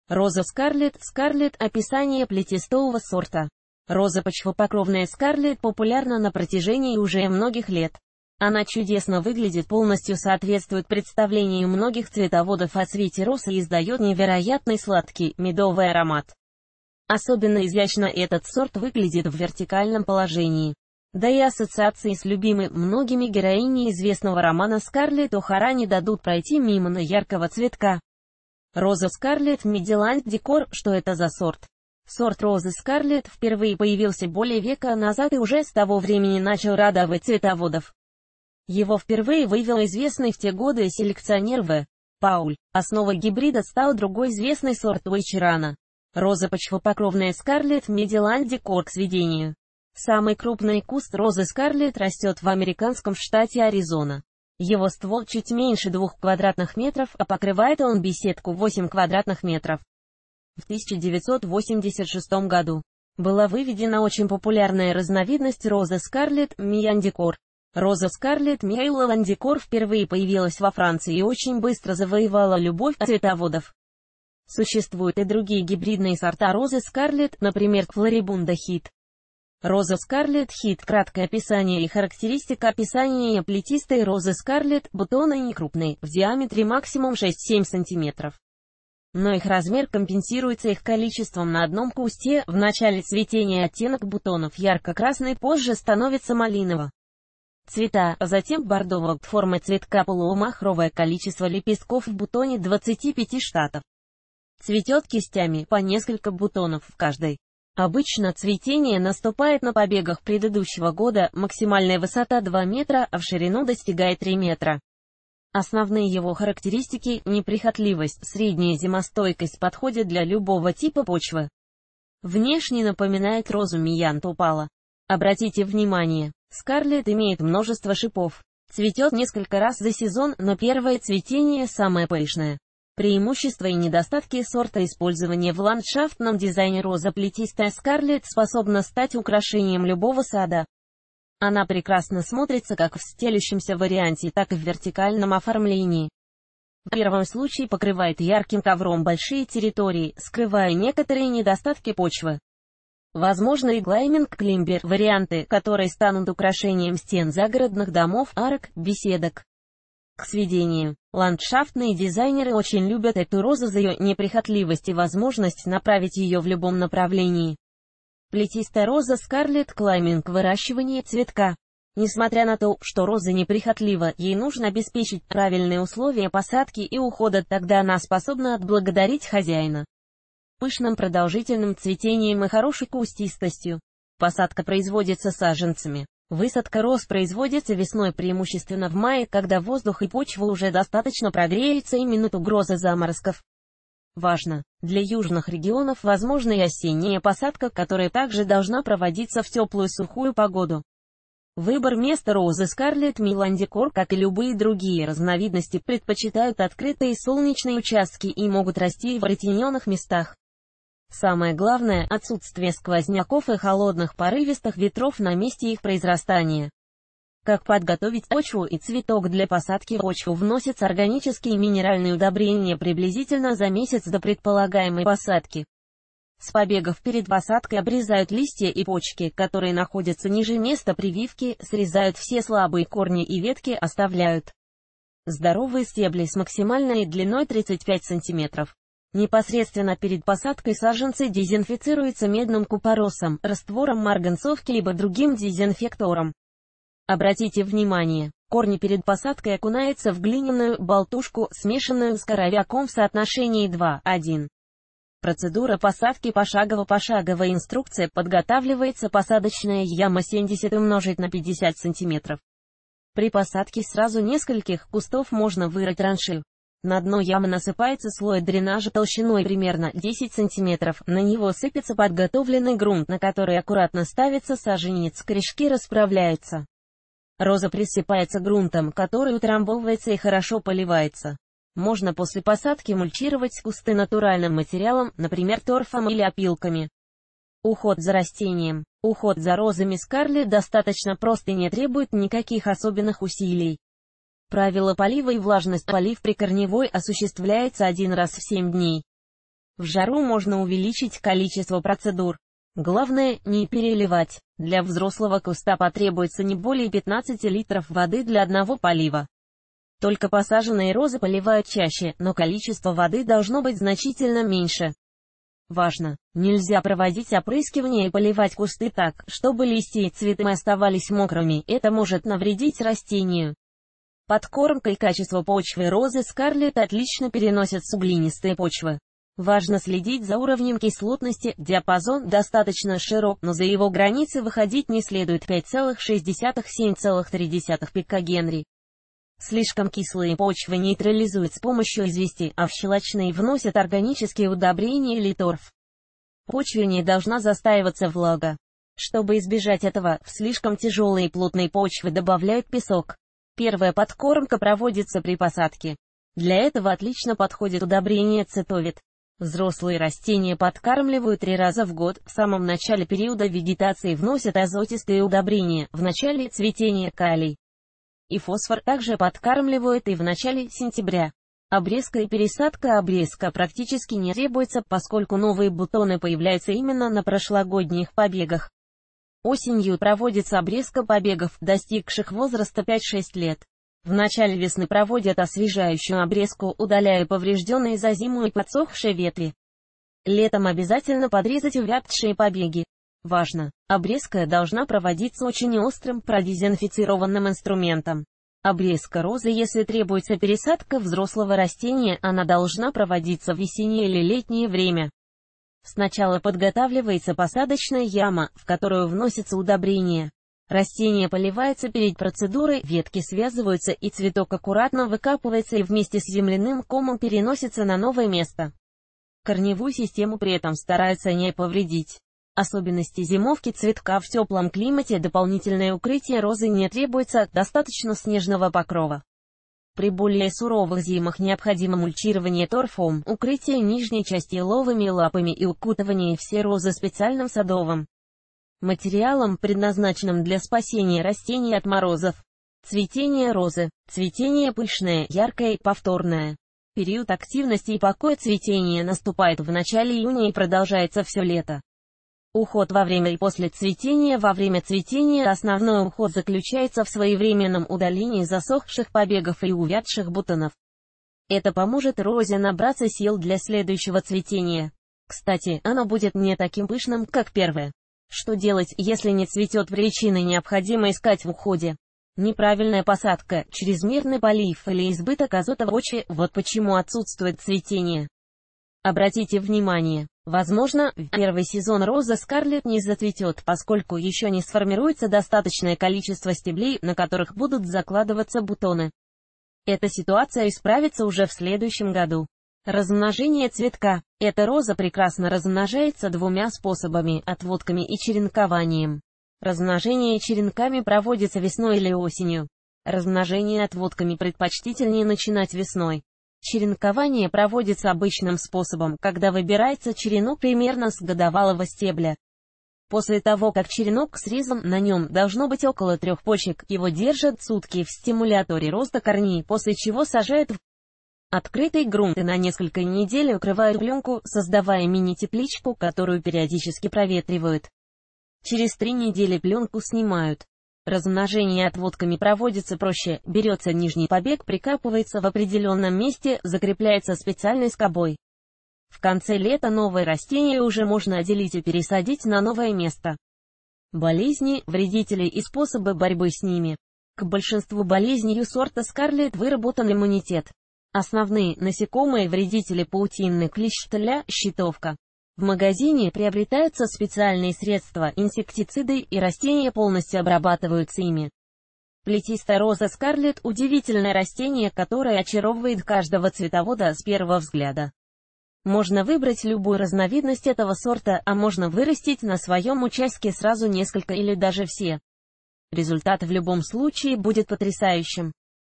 Роза Скарлет (Scarlet) — описание плетистого сорта
Роза Скарлет Мейдиланд Декор (Scarlet Meidiland Decor) — что это за почвопокровный сорт, описание, как выглядит. Особенности выращивания плетистого шраба, как посадить розовый куст, правила ухода для наступления периода цветения.